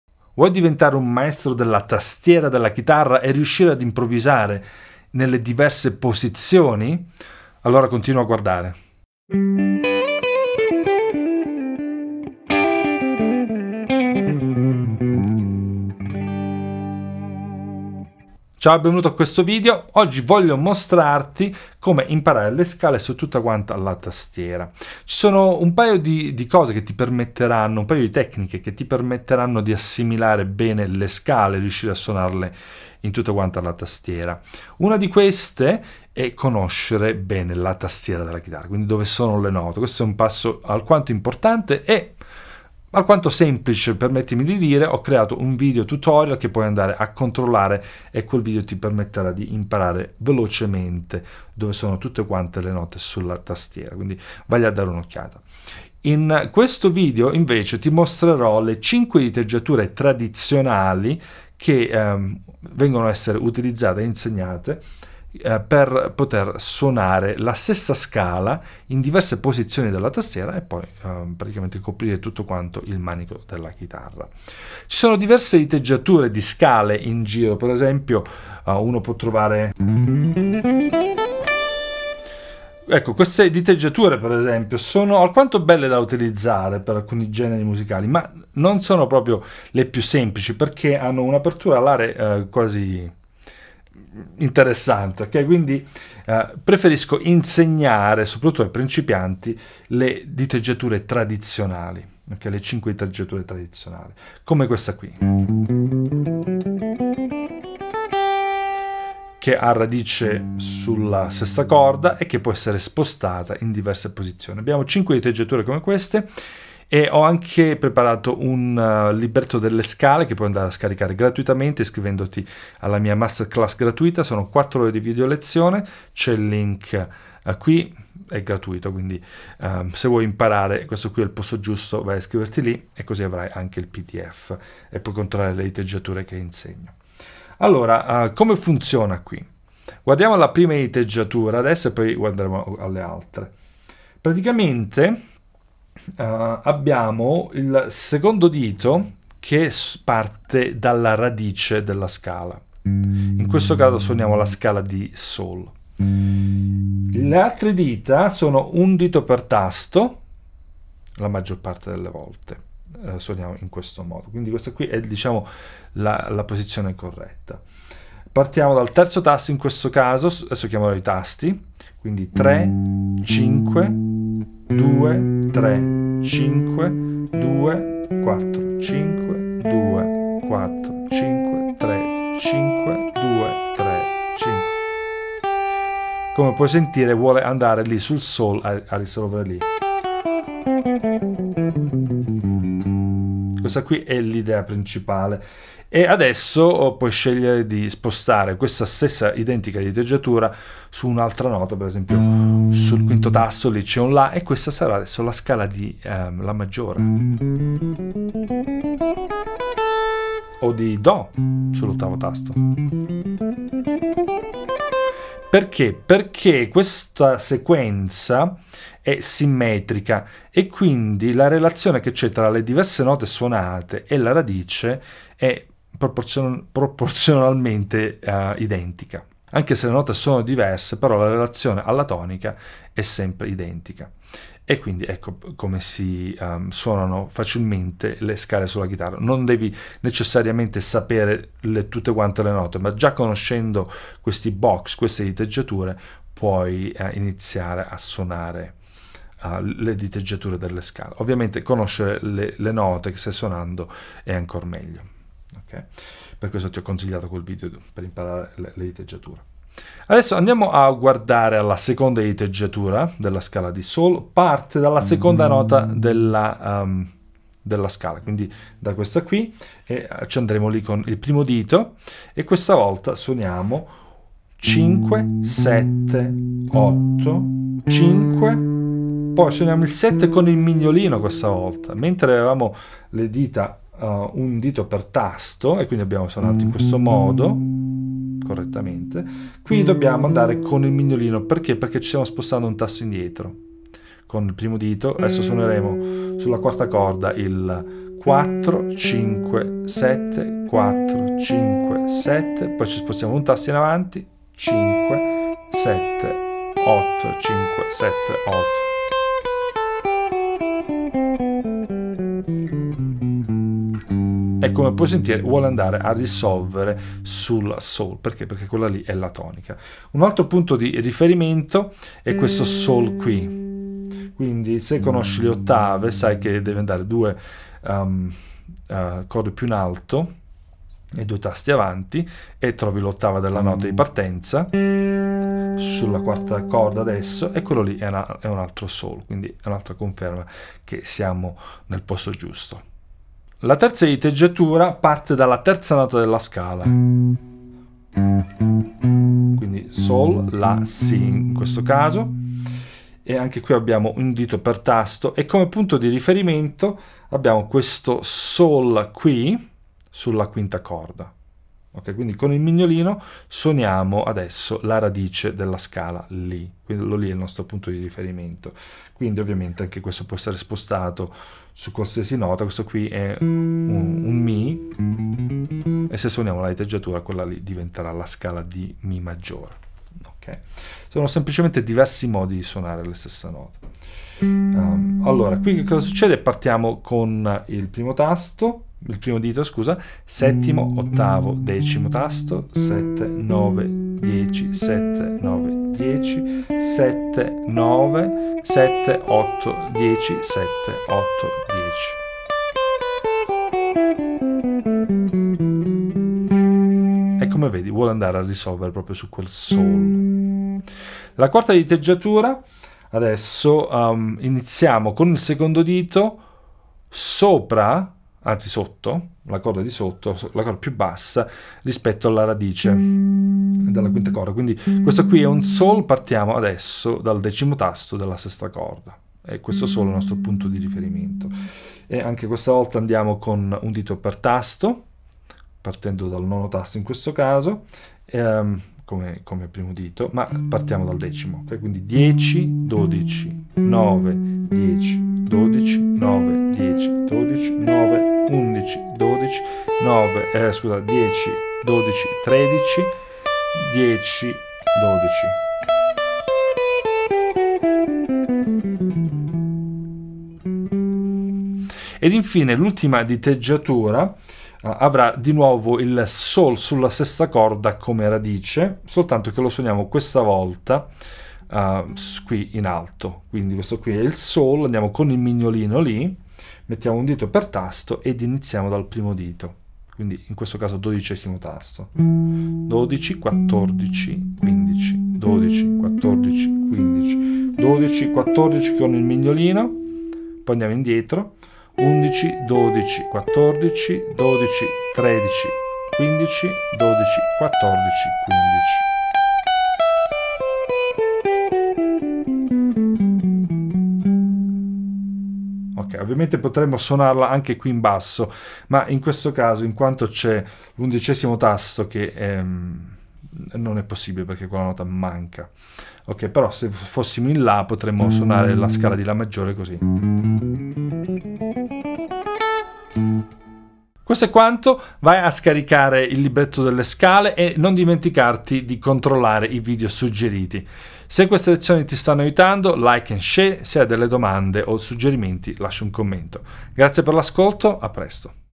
5 Diteggiature Indispensabili Della Scala Maggiore
Conoscere la scala maggiore in tutte le posizioni del manico della chitarra è molto importante per ogni chitarrista che voglia imparare ad improvvisare. In questo video ti mostrerò 5 diteggiature indispensabili.